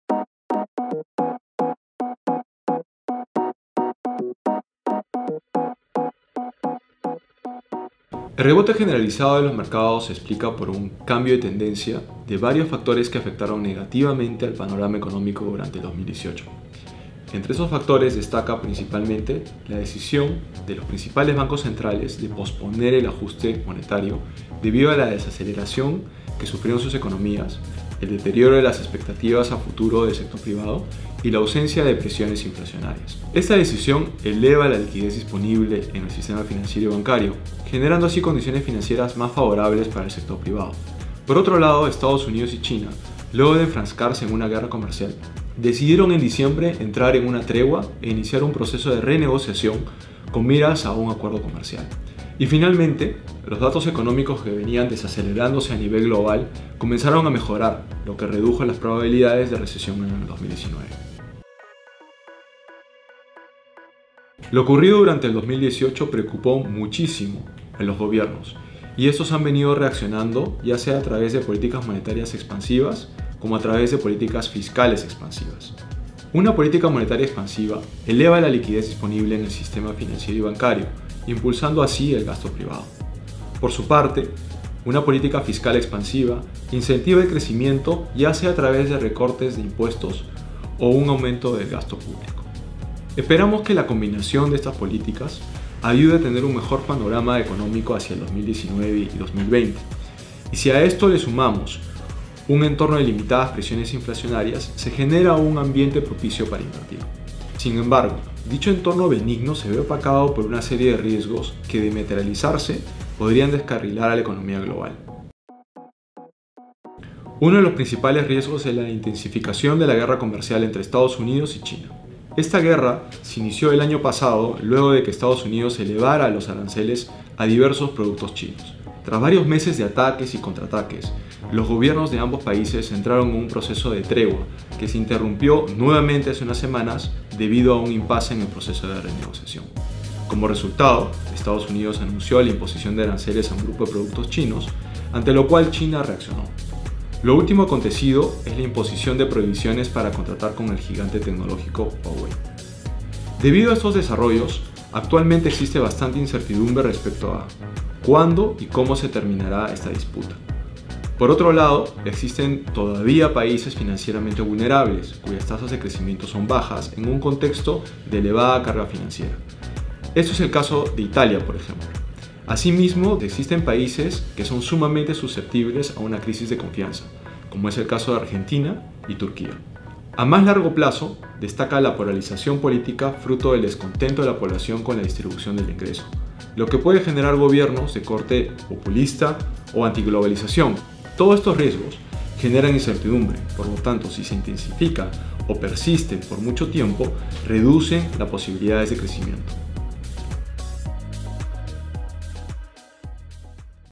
[Prima AFP] Situación de los mercados financieros en el 2019.
Gonzalo Llosa, Gerente de Estrategia de Inversión de Prima AFP, explica qué factores influyeron en el rebote de los mercados financieros de enero a abril del 2019 y cuál es el panorama económico y los riesgos para los próximos meses.

Gonzalo es Doctor y Máster en Economía por la Universidad de California en Los Angeles (UCLA) y Bachiller de Economía por la Universidad del Pacífico. Ha sido Consultor del Banco Interamericano de Desarrollo y Economista del Banco Central de Reserva del Perú.